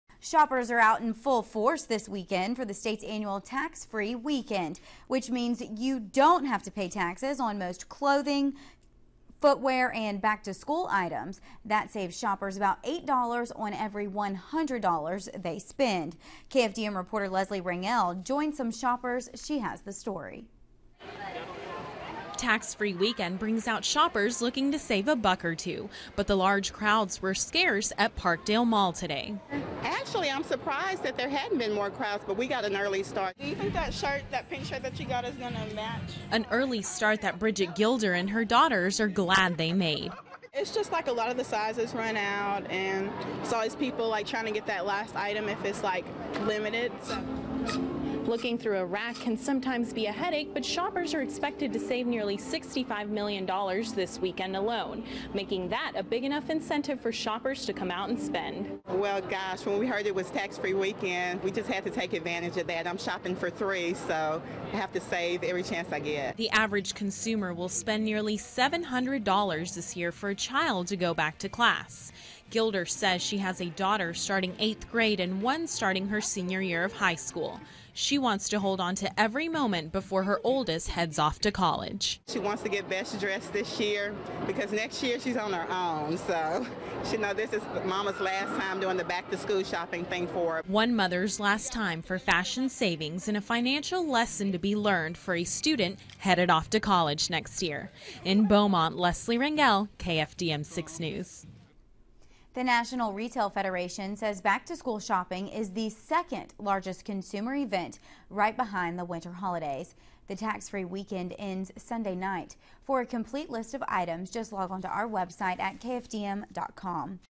Tax Free weekend kicks off the back-to-school season
BEAUMONT-Shoppers are out in full force this weekend for the state's annual tax-free weekend. This means you don't have to pay taxes on most clothing, footwear, and back to school items. That saves shoppers about  on every 00 they spend. Shoppers can benefit from tax breaks on most school supplies and clothing priced under one hundred dollars.